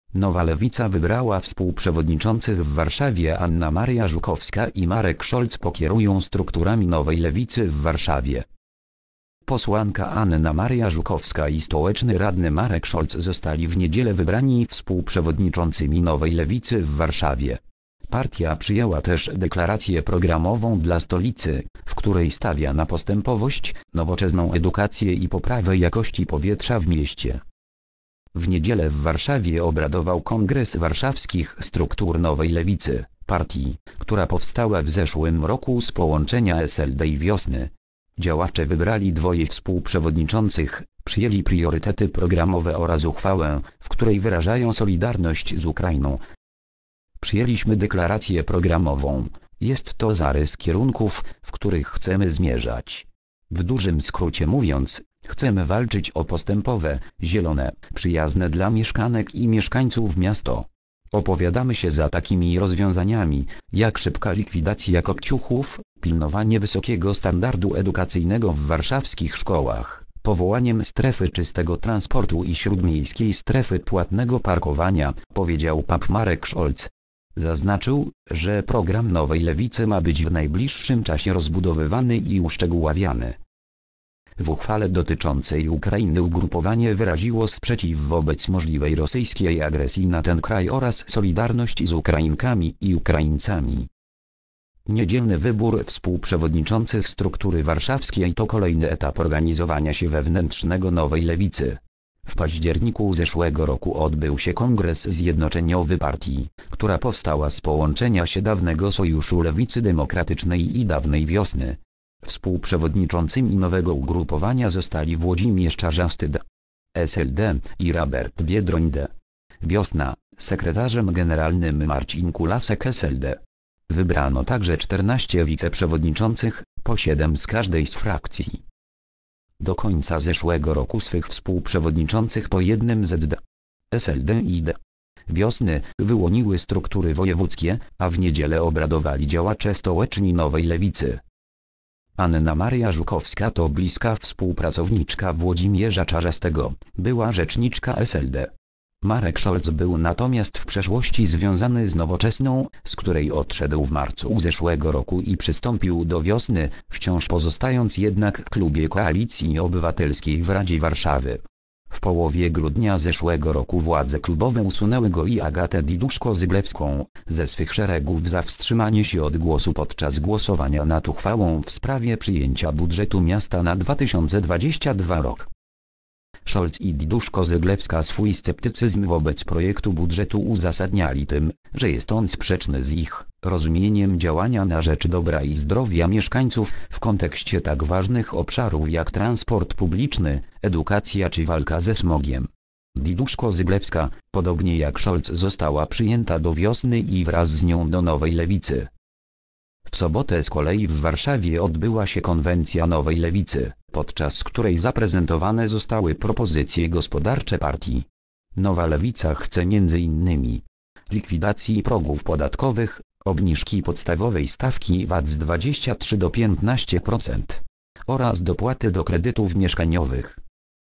Nowa Lewica wybrała współprzewodniczących w Warszawie.
Anna Maria Żukowska i Marek Szolc pokierują strukturami Nowej Lewicy w Warszawie.

   (fot. PAP/Mateusz Marek)